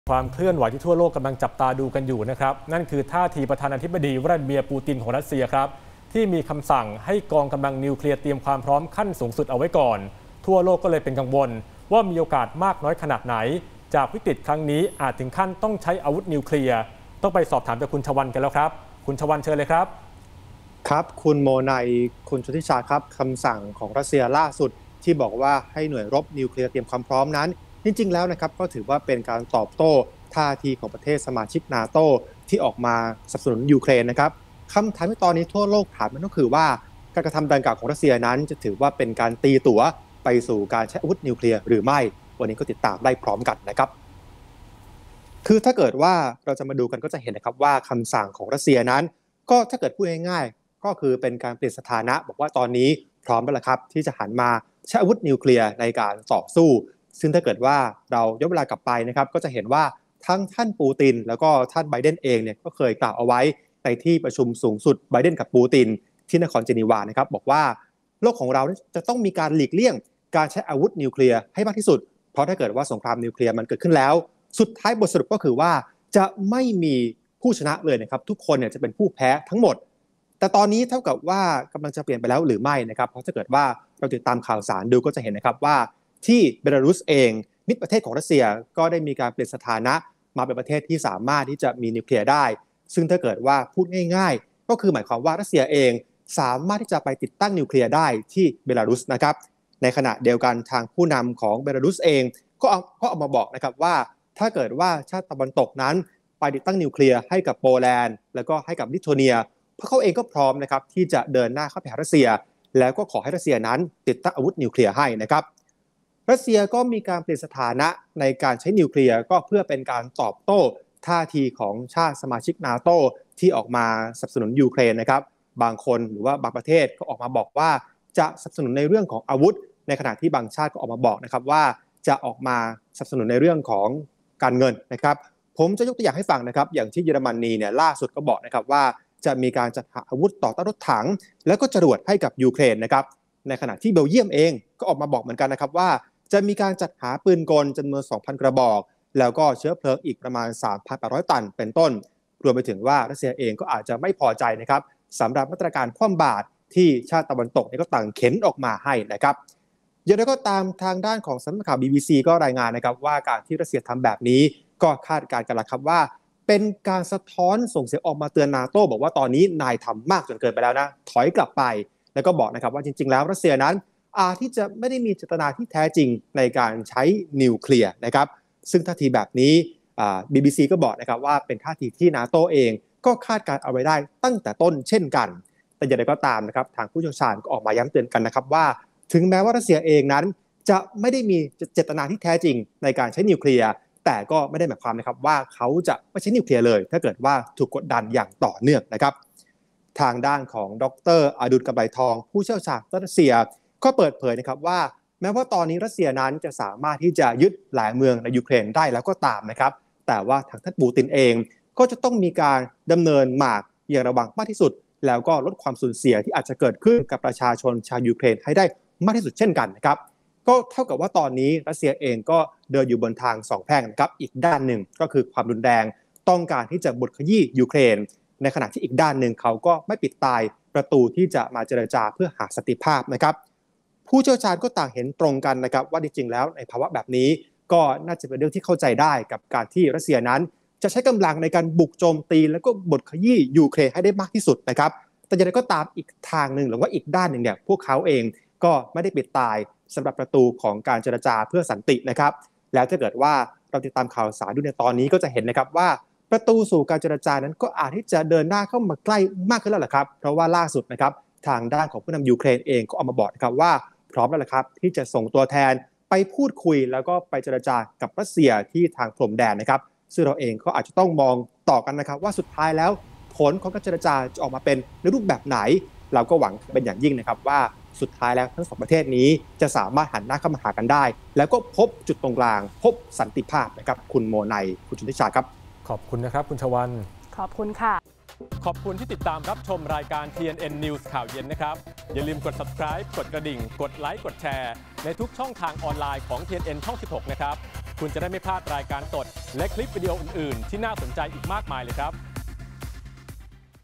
รัสเซียเตรียม "นิวเคลียร์" อ่านใจ "ปูติน" เอาจริงหรือแค่เตือน | TrueVroom | TNN ข่าวเย็น | 28-02-22
การเผชิญหน้าระหว่างรัสเซียกับยูเครนยังต้องจับตามองกันอย่างใกล้ชิด หลังประธานิธิบดีวลาดีมีร์ ปูติน ได้สั่งการให้หน่วยรบนิวเคลียร์เตรียมความพร้อม จนหลายฝ่ายกังวลว่าอาจจะเกิดการใช้นิวเคลียร์ขึ้นจริง โอกาสความเป็นไปได้มีมากน้อยแค่ไหน ติดตามความคืบหน้าจากคุณ ชวัลน์ จันทร์ทรัพย์